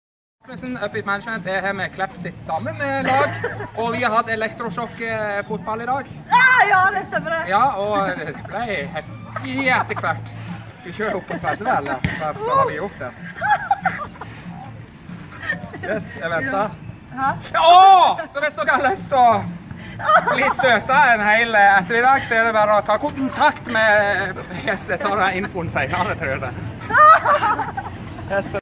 Elektrosjokkfotball m/Klepp Damelag (Electroshock Football/Soccer)
Her tar vi prat med Line fra Klepp sitt damelag. Lite prat, litt vondt men utroligt gøy!
Elektrosjokk fungerer til alle sportsgrener.